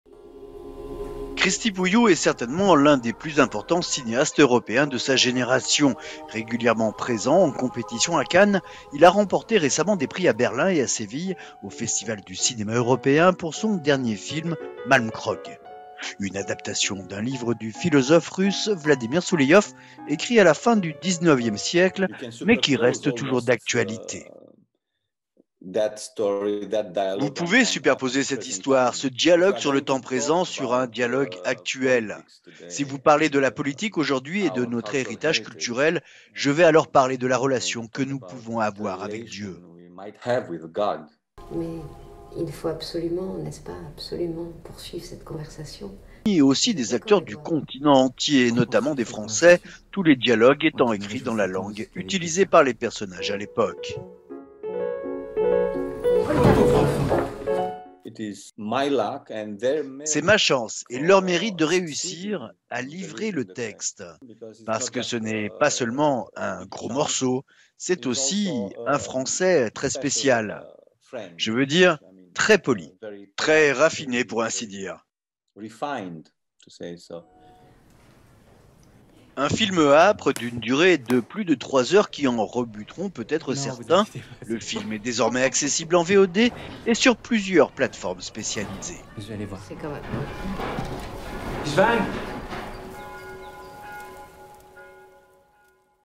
Cristi Puiu signe un film fleuve philosophique, primé dans de nombreux festivals
Avec Malmkrog, le cinéaste roumain Cristi Puiu adapte un texte russe du XIXème siècle, et livre une réflexion sur l'homme qui traverse les époques.